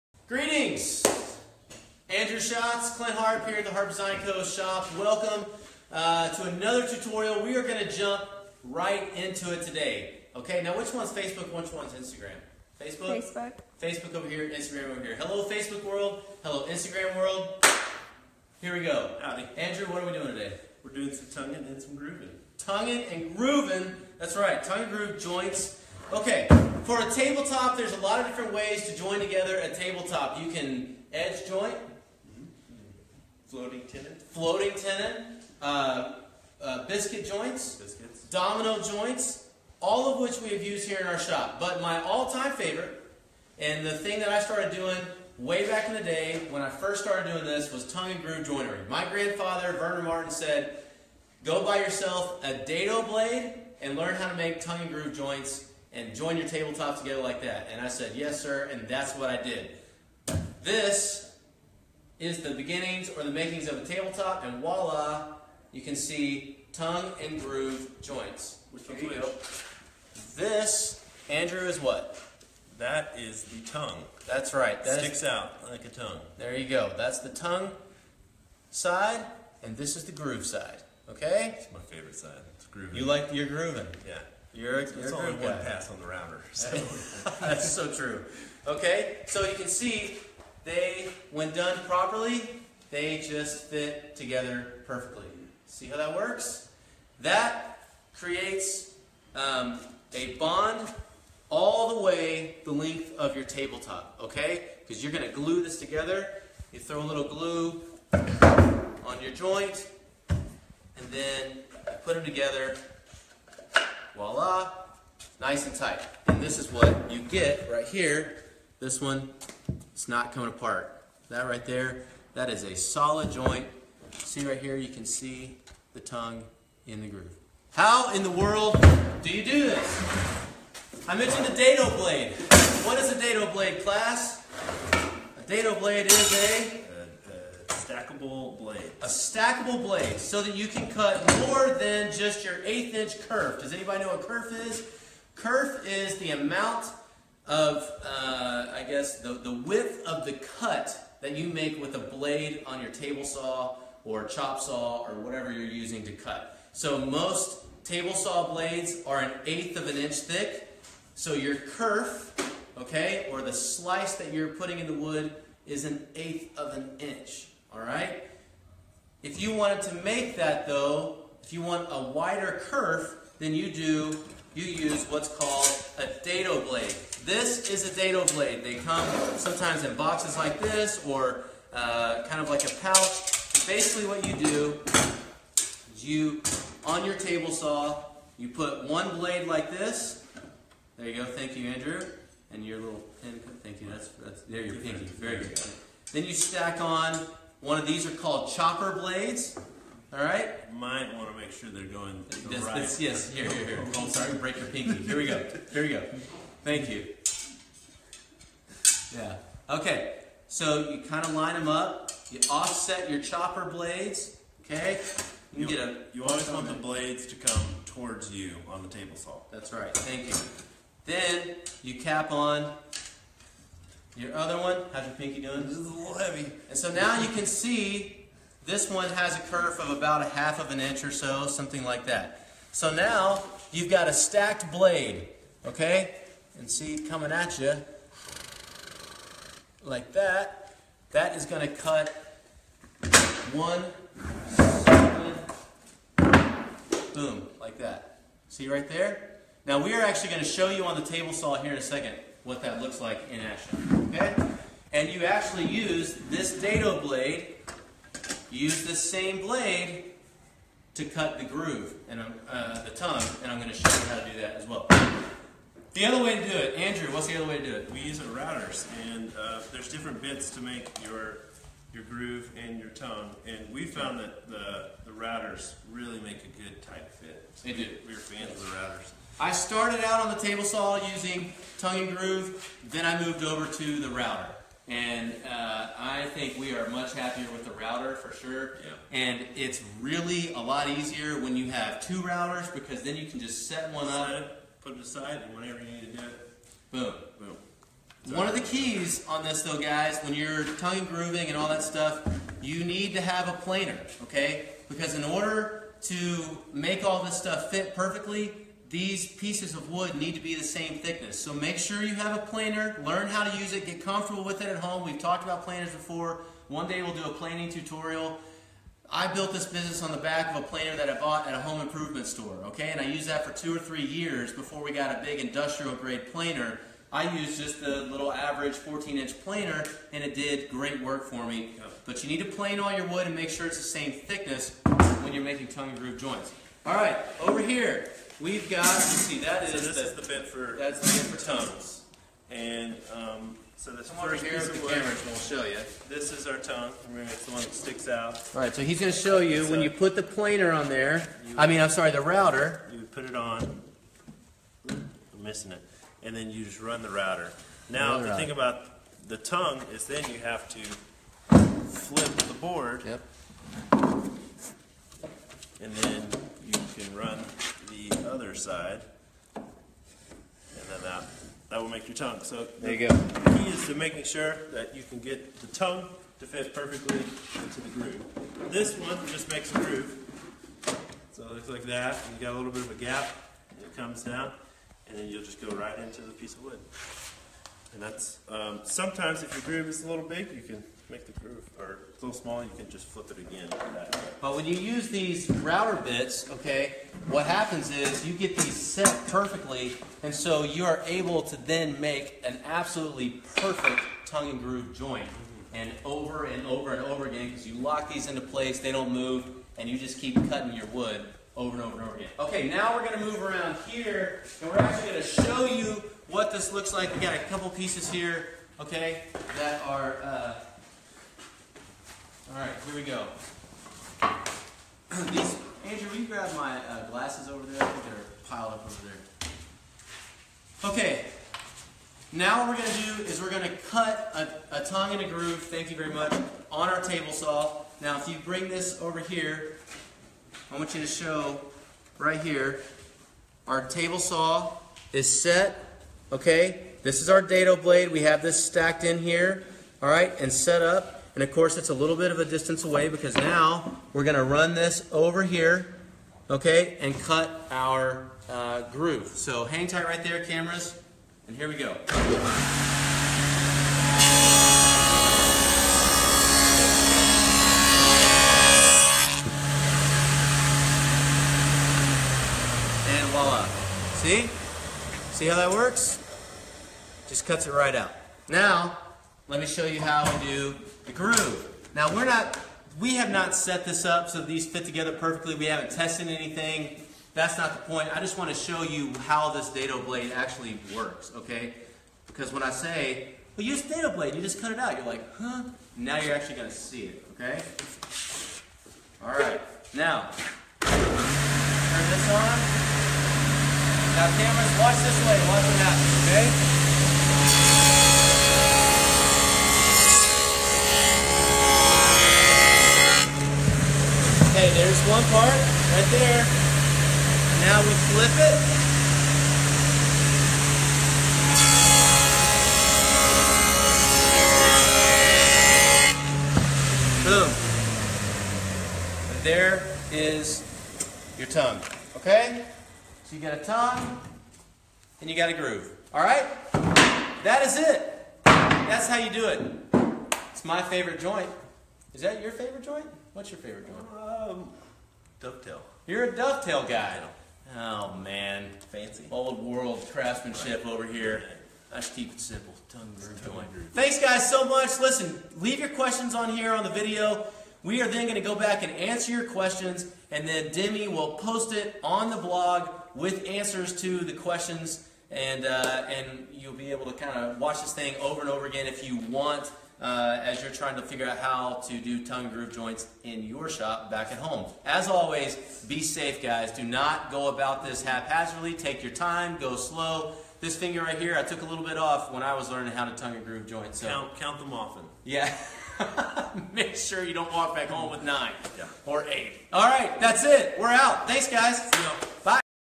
How to Make a Tongue-and-Groove Joint with Harp Design Co.
Tongue and groove joinery is Clint Harp's favorite method when joining table tops – this how to make tongue and groove tutorial is one you’ll definitely want to check out!